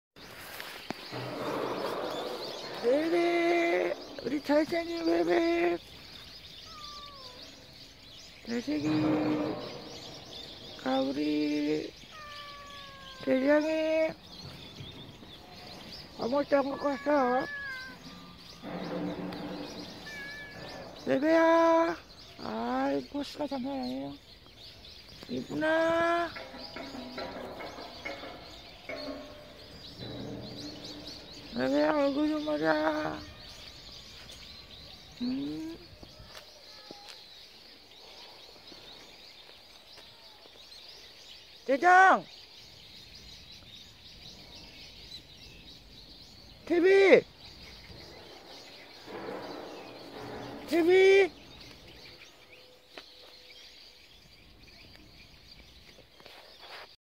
길냥이대장이,집냥이베베!// The stray cat Dajangee,house cat Bebe.
베베는 미모로도 top이지만,체구도 엄청커요,대장이가 매일 싸움걸드니,베베가 싸워나보네요,넘순해요 우리베베,시크하고,사람을더조아하지요!// Bebe'beauty is the top in district,he has a huge figure.Dajangee tries to  fight  with Bebe everyday,but Bebe always avoids Da jangee,but I think Bebe finally fighting  today with Dajangee.Bebe is too gentle,chic. He likes people more than cats.